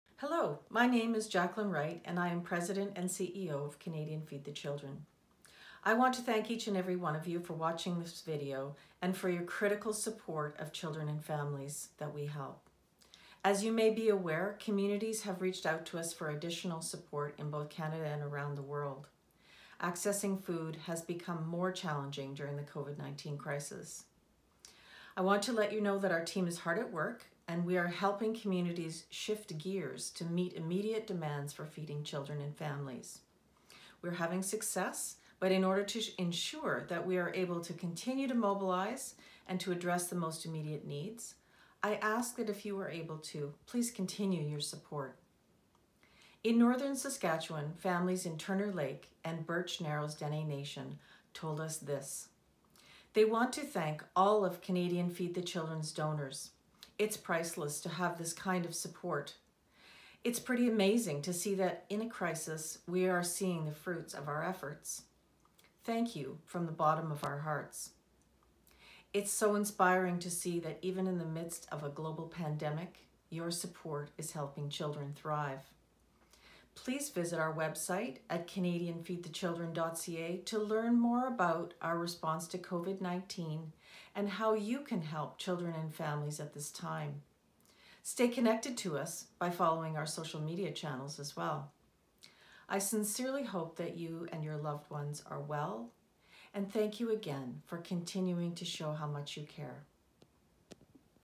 A message from Jacquelyn Wright, President & CEO
We want to thank everyone for their continued support at this uncertain time as we shift to helping communities adapt to new challenges.